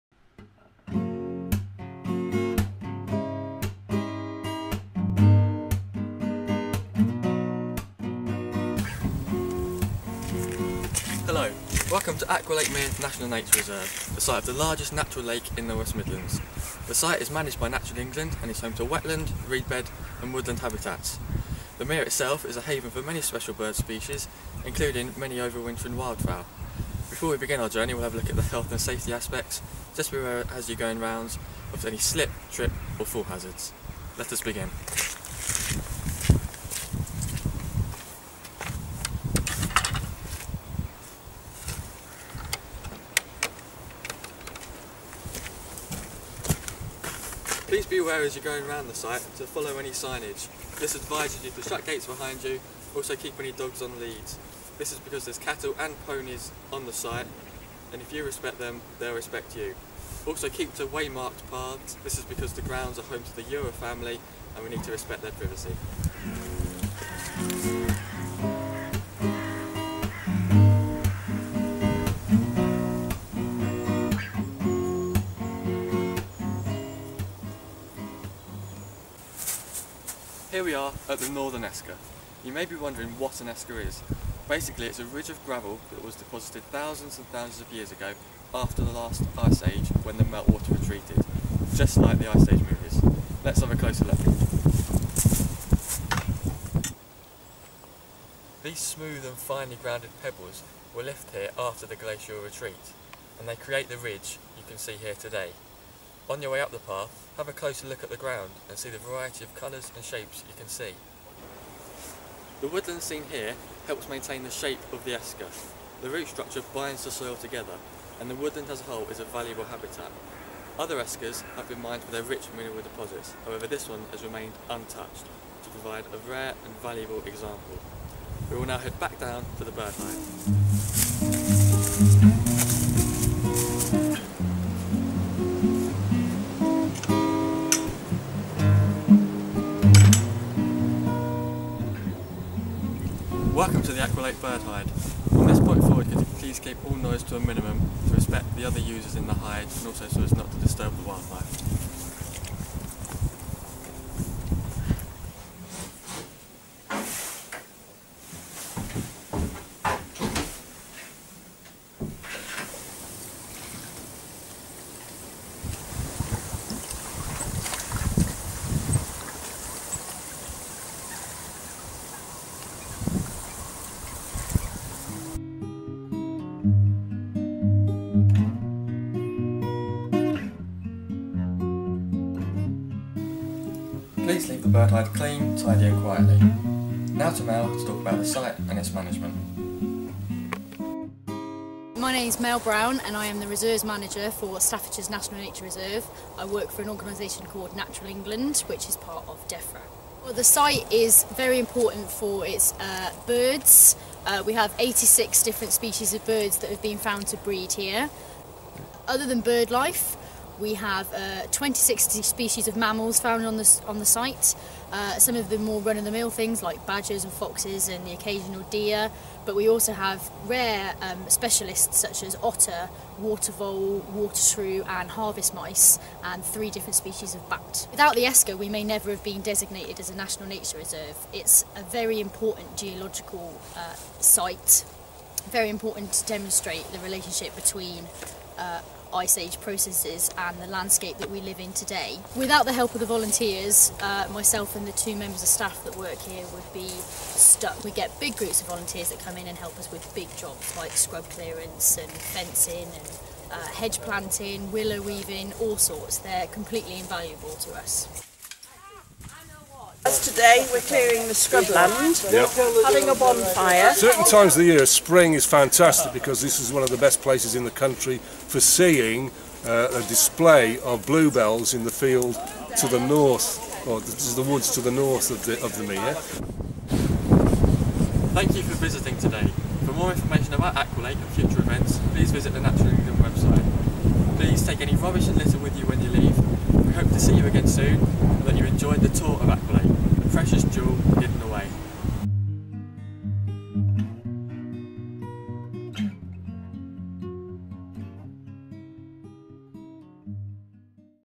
Aqualate Mere Virtual Tour
A short video to provide an alternative to, or to complement a visit to Aqualate Mere National Nature Reserve.